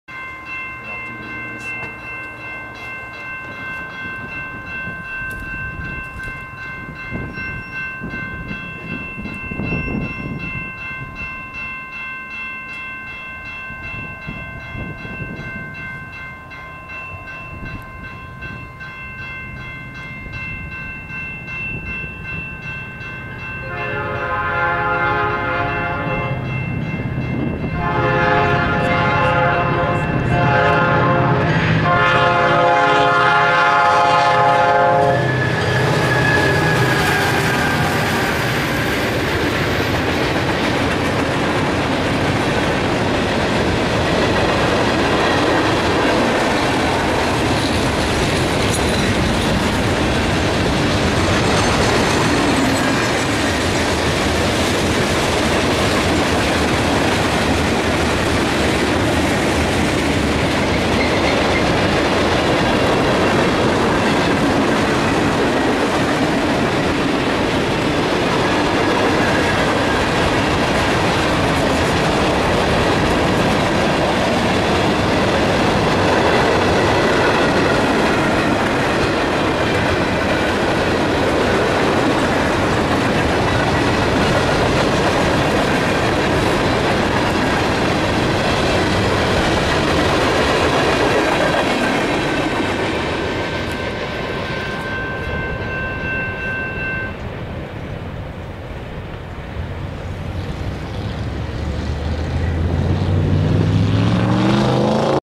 DME 6086 & 6087 Leads CP 277 West, Sturtevant, WI
I'll take a pair of red DME's!

CP C&M Subdivision
Filmed 1/7/12